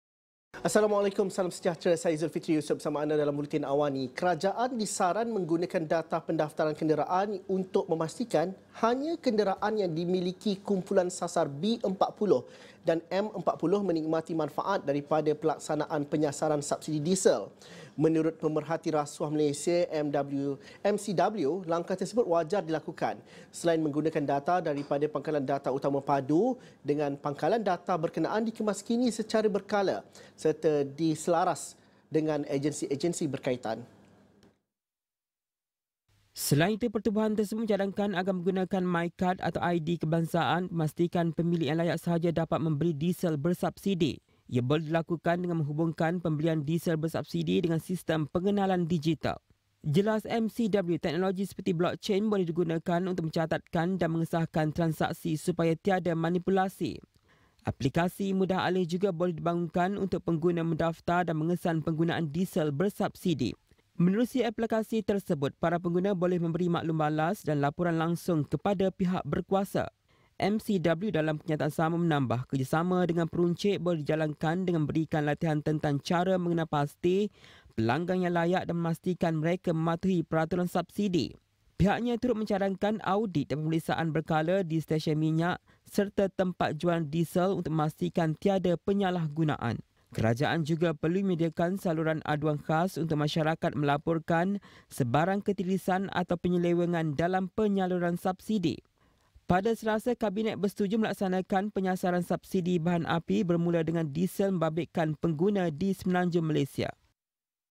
MCW cadang guna data pendaftaran kenderaan milik B40, M40
Selain itu, MCW mencadangkan menggunakan MyKad atau ID kebangsaan untuk memastikan pemilik yang layak sahaja dapat membeli diesel bersubsidi.